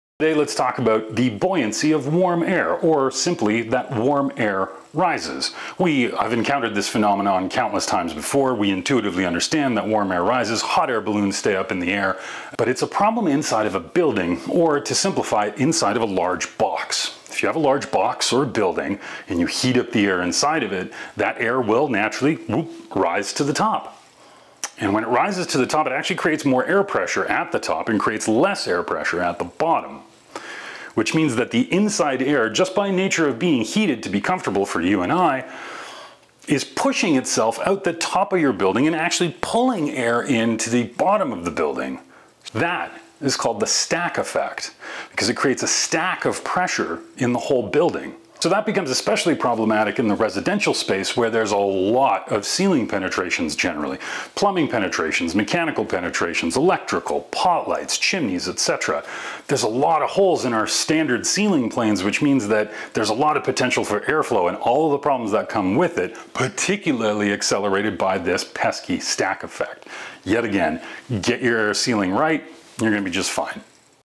Building Science Crash Course - Ep 14 - The Stack Effect
Last time we looked at how wind is a jerk and manages to push air in and out of the building at the same time. But as it turns out our buildings are also jerks and make their OWN wind inside. So to speak. The stack effect is the result of the buoyancy of warm air pushing air up to the top of a building and it underscores yet again the importance of needing to get your air tightness JUST right.